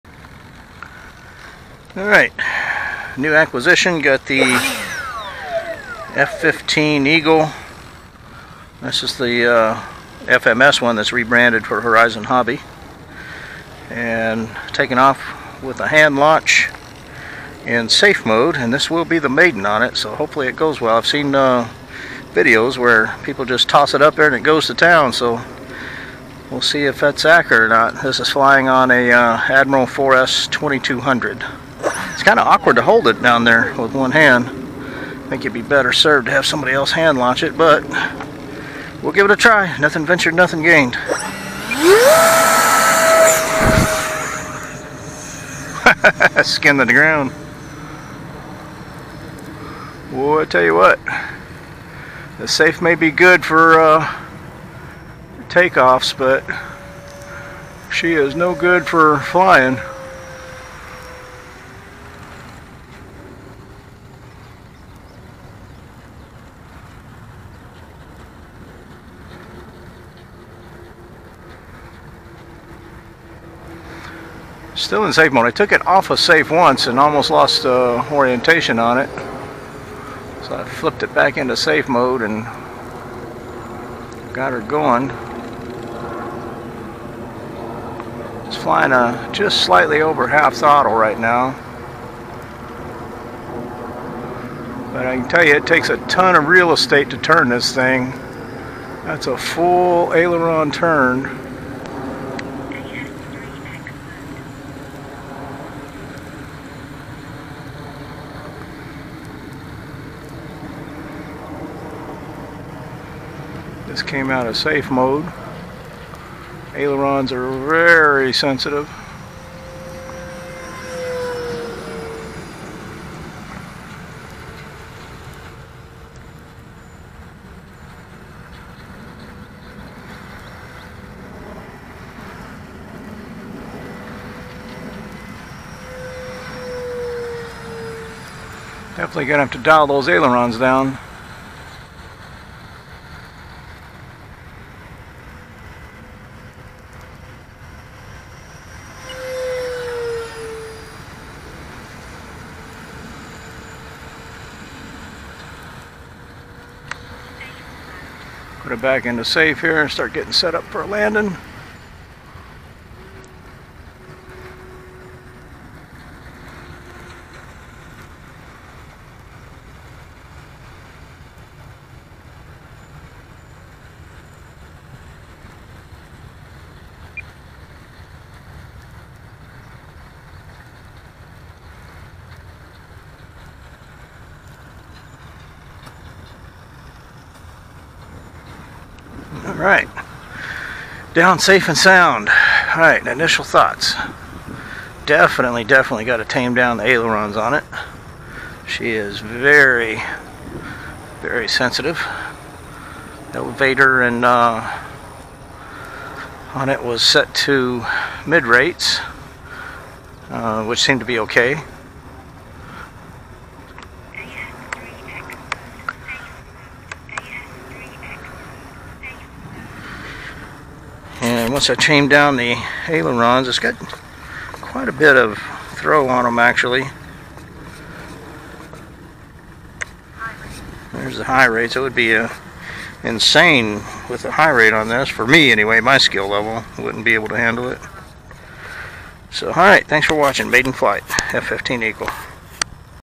eFlite 64mm F-15 Maiden Flight on 7-10-19
This is the maiden flight of my 64mm eFlite F-15

Flying on a 4s, 2200mAh battery. I had my ailerons in low rates, which made the turns incredibly wide when the SAFE mode was activated. (Needs to be in hi-rates while SAFE is active)